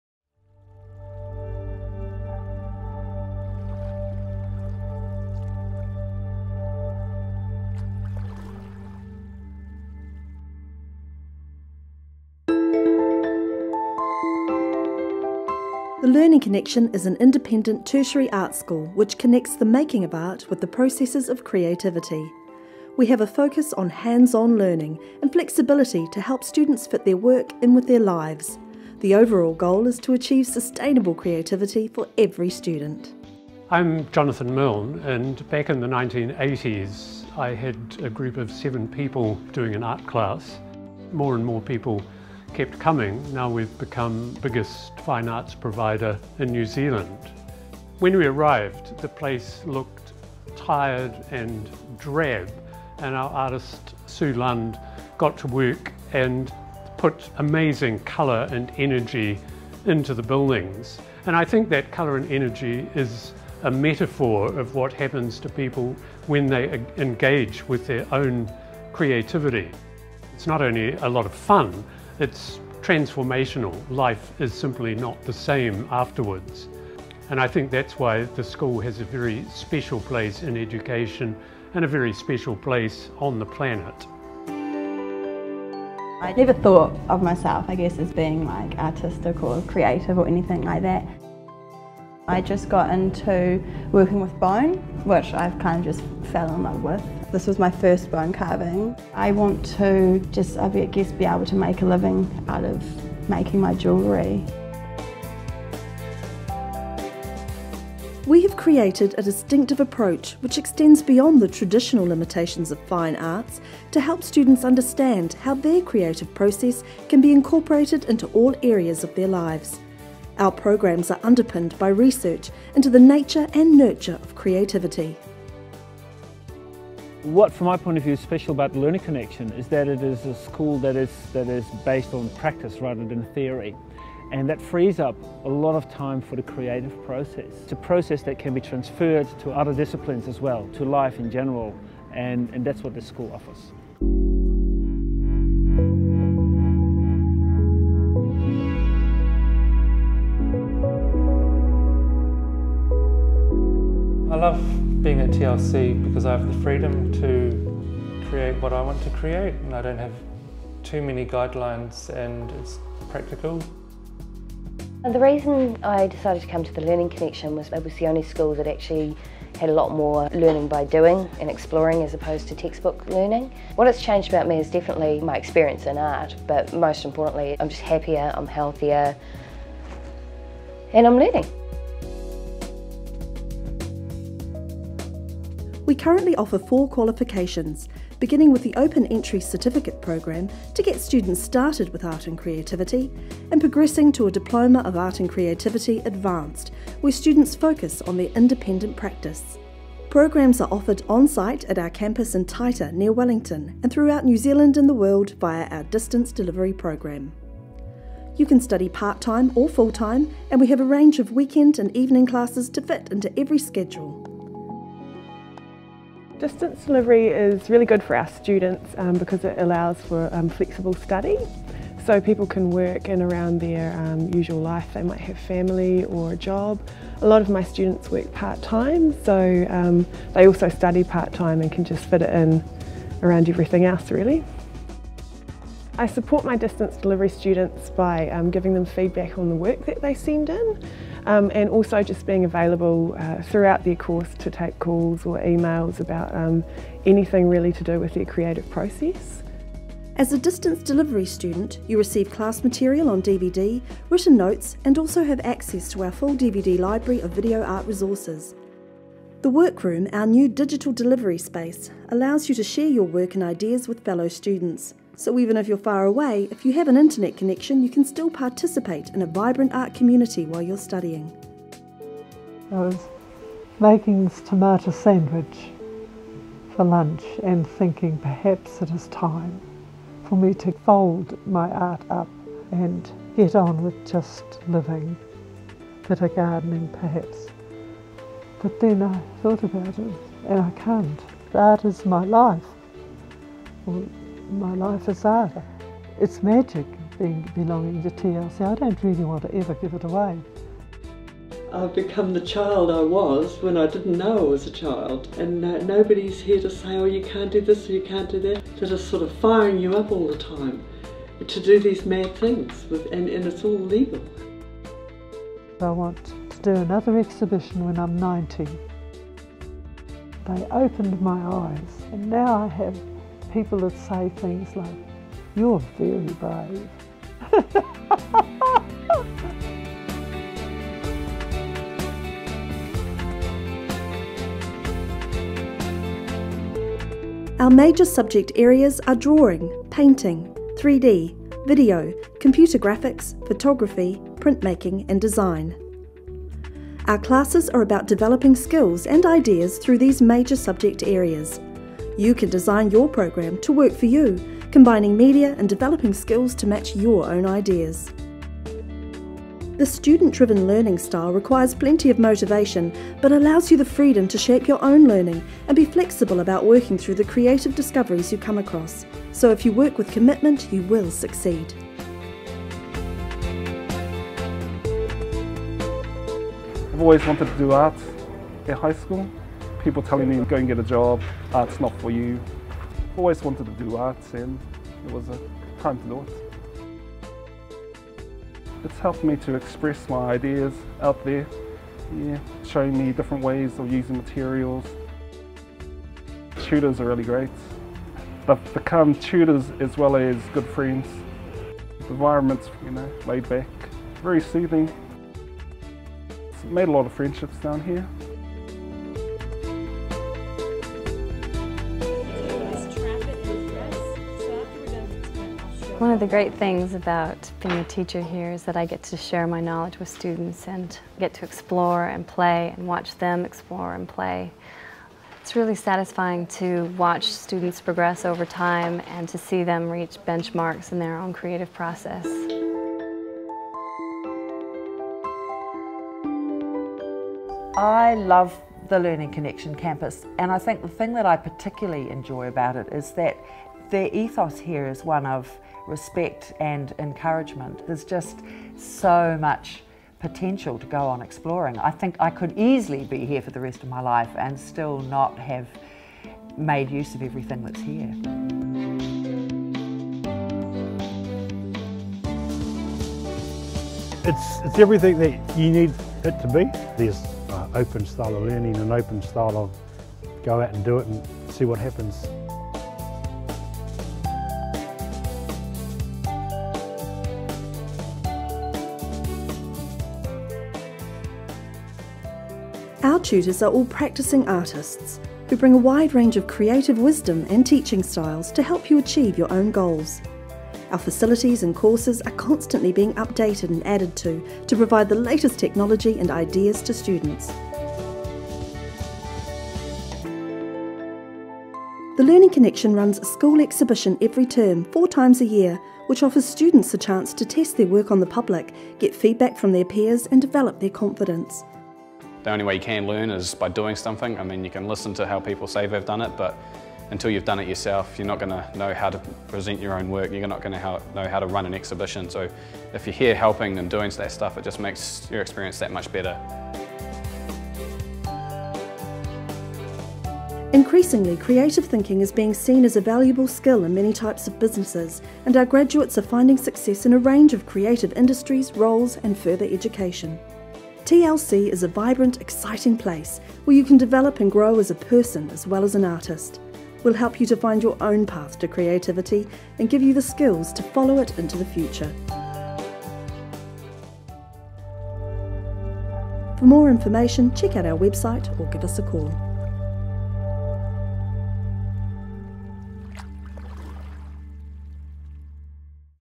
Experience The Learning Connexion
The Learning Connexion teaches sustainable creativity through art making. We are a tertiary art and creativity school with facilities across all fine and contemporary art mediums from video to bronze-casting, computers to painting.

The overall goal of our course is to achieve sustainable creativity for every student. Anyone can study at The Learning Connexion. We offer a university level, NZQA approved course the key requirement is commitment.